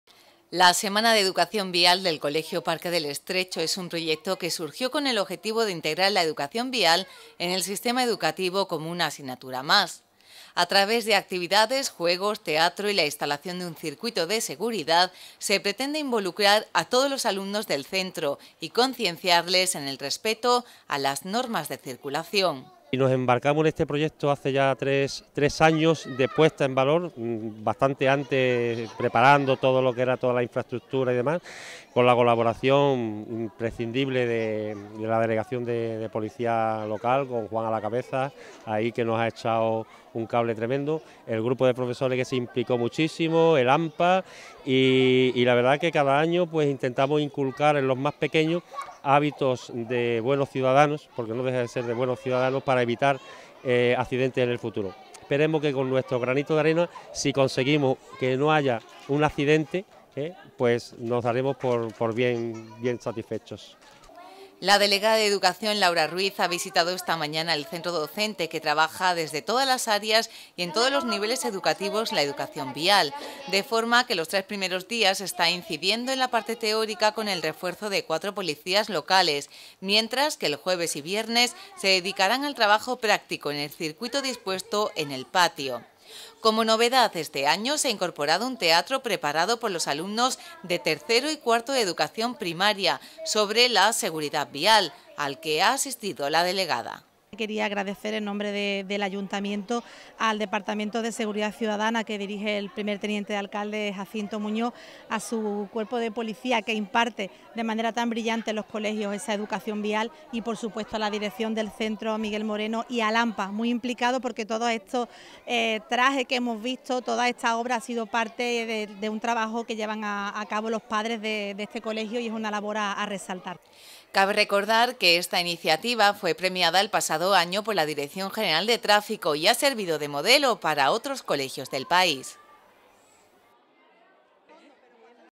El Colegio Parque del Estrecho celebra una nueva edición de su “Semana de Educación Vial”.
Un año más el colegio de Educación Infantil y Primaria Parque del Estrecho celebra su Semana de la Educación Vial. Una iniciativa que el pasado año fué premiada por la dirección general de Tráfico y que ha servido de modelo para otros colegios del país.